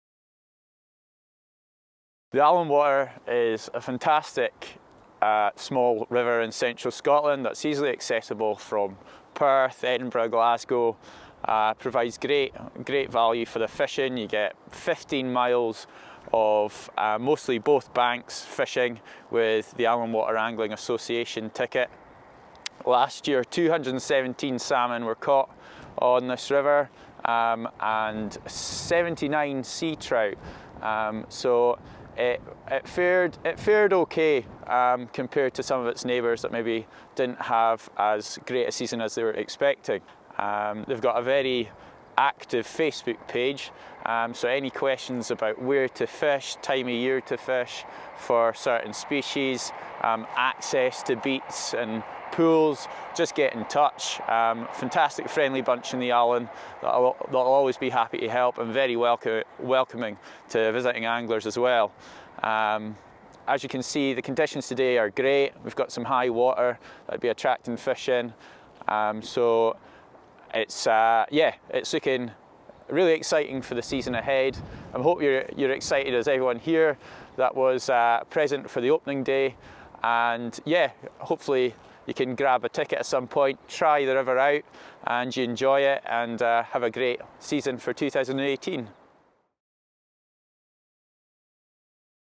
Part 1 - The River, 2018 River Allan Opening
The River Allan fishing opens annually on the 15th March in Kinbuck. The day brought a few new and existing members together.